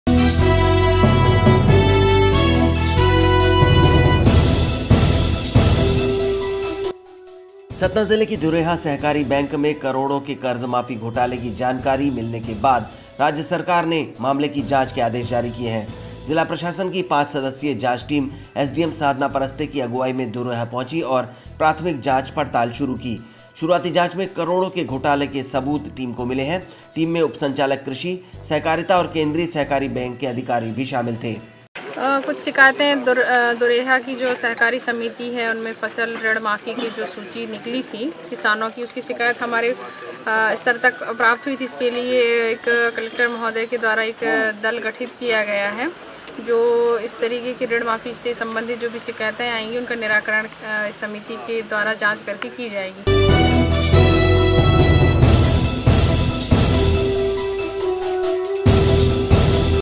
सतना में सहकारी बैंक घोटाले की जांच शुरू
सतना जिले की दुरेहा सहकारी बैंक में करोड़ों के कर्ज माफी घोटाले की जानकारी मिलने के बाद राज्य सरकार ने मामले की जांच के आदेश जारी किये हैं, जिला प्रशासन की पांच सदस्यीय जांच टीम एसडीएम साधना परस्ते की अगुआई में दुरेहा पहुची और प्राथमिक जांच पड़ताल शुरू की। शुरुआती जाँच में करोड़ो के घोटाले के सबूत टीम को मिले हैं। टीम में उप संचालक कृषी ,सहकारिता और क्रेंदीय सहकारी बैंक के अधिकारी भी शामिल थे,